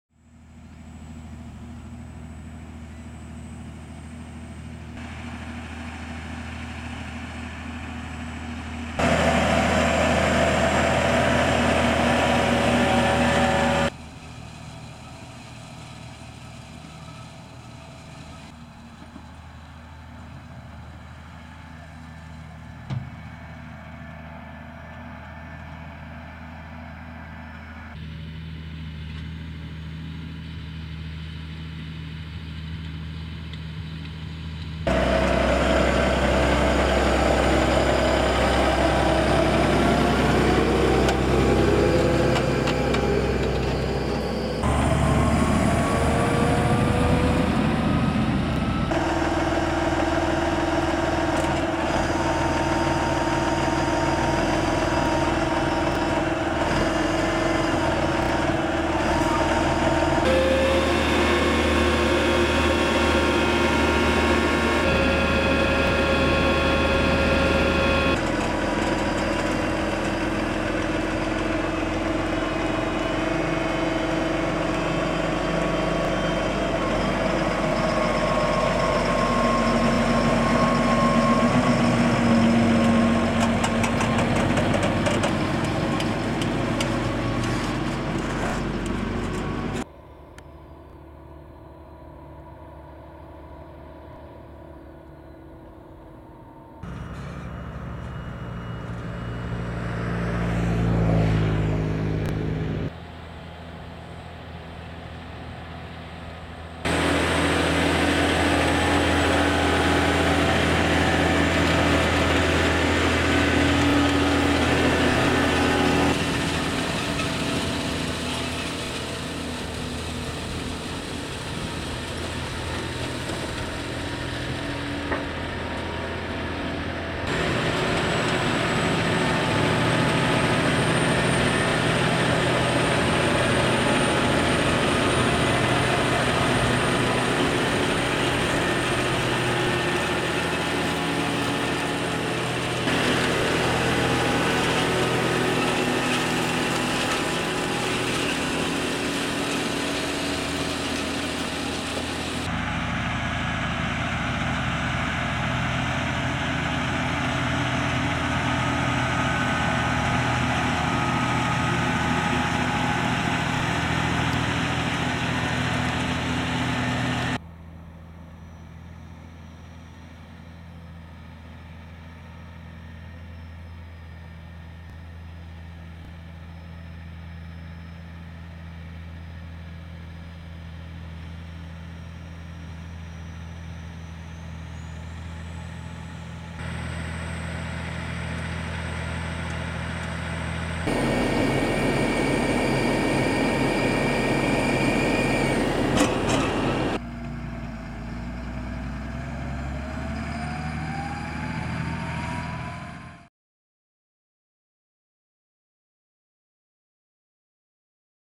Zodebemesten met Massey Ferguson & Schuitemaker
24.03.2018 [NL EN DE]
Zodebemesten met de Massey Ferguson 8690 trekker, Schuitemaker Robusta mesttank en Vredo zodebemester.

Slurry injection using the Massey Ferguson 8690 tractor, Schuitemaker Robusta slurry tanker and Vredo slurry injector.

Gülle fahren mit der Massey Ferguson 8690 Traktor, Schuitemaker Robusta Güllefaß und Vredo Gülleschlitzgerät.